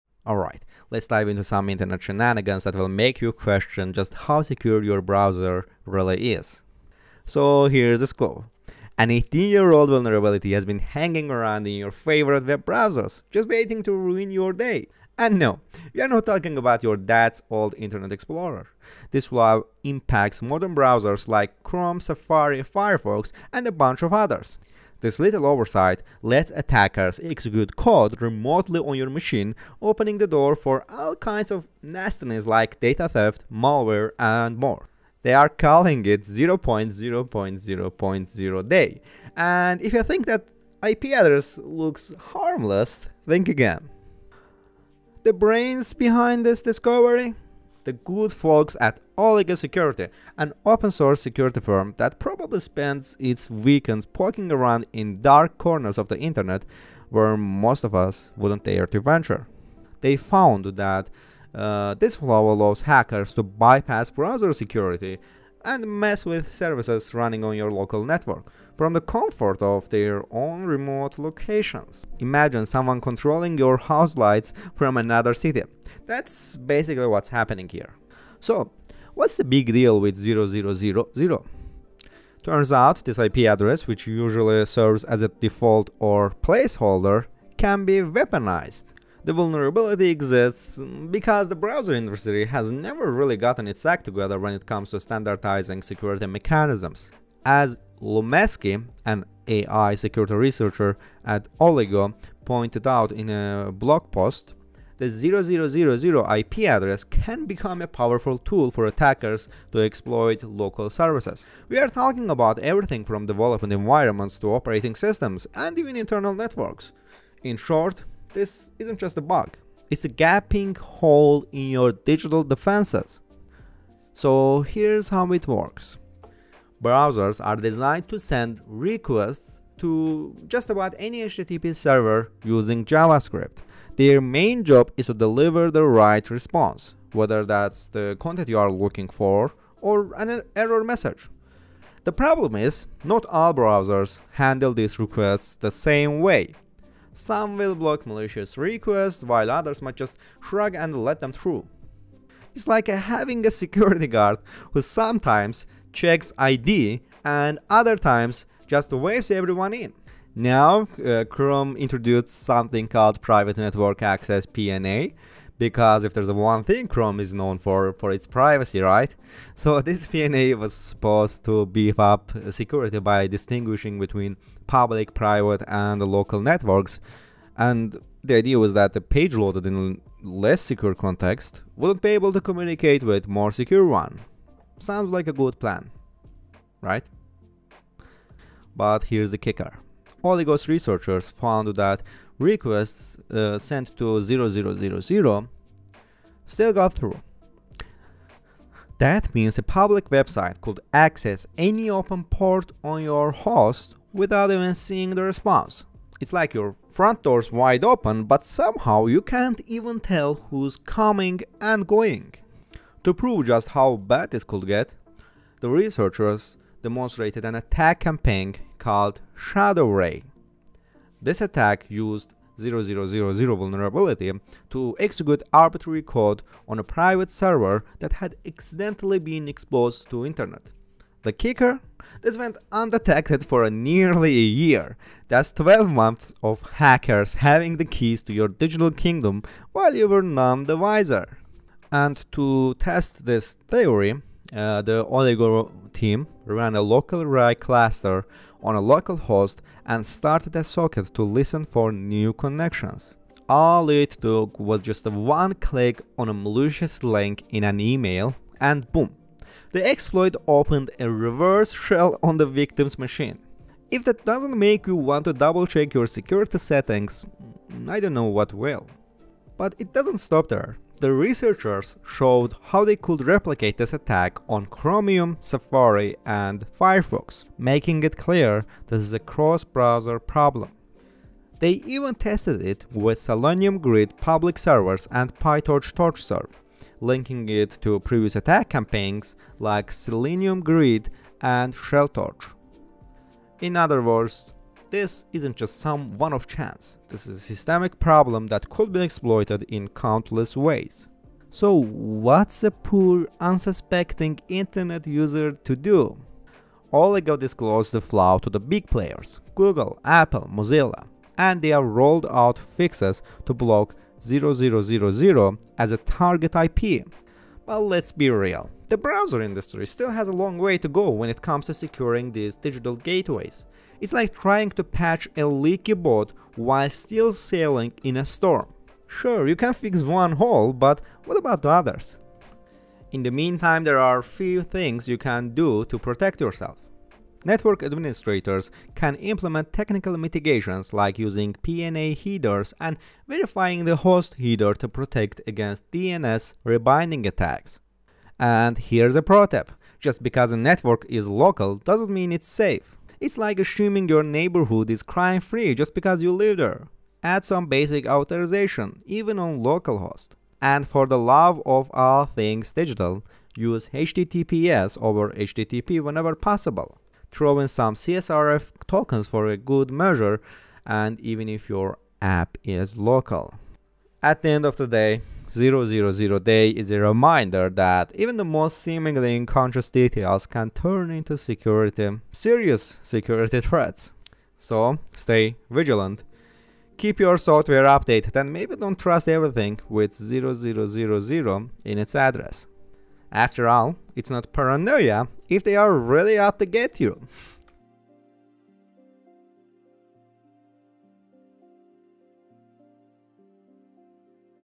0.0.0.0 Day: The 18-Year-Old Browser Flaw That Makes Privacy a Joke
Welcome to another deep dive into the digital world where today we’re uncovering a serious security flaw that’s been lurking in our browsers for nearly two decades!

🔍 What’s the Deal?
A newly discovered vulnerability, affectionately dubbed “0.0.0.0 Day,” impacts major browsers like Chrome, Safari, and Firefox. This flaw lets attackers remotely execute code on your system by exploiting an IP address that sounds more like a placeholder than a security threat. Yes, you read that right—this could lead to data theft, malware, and other malicious activities.

This vulnerability lets hackers bypass browser security and interact with your local network from afar. Imagine someone being able to control your local services just by visiting a website. Sounds like a sci-fi horror story? Unfortunately, it’s very real.

🔒 Chrome’s ‘Private’ Mode—Not So Private?
Chrome introduced Private Network Access (PNA) to tackle such issues, but let’s be real: if you trust Chrome’s “Private” mode to keep your data secure, you might want to think again. After all, it’s the same browser that’s mastered the art of pretending to be private while collecting every bit of your data.

We break down the details of how this vulnerability works, the real-world implications, and what steps you can take to protect yourself. From implementing PNA headers to using HTTPS and CSRF tokens, we’ve got practical tips to help you stay secure.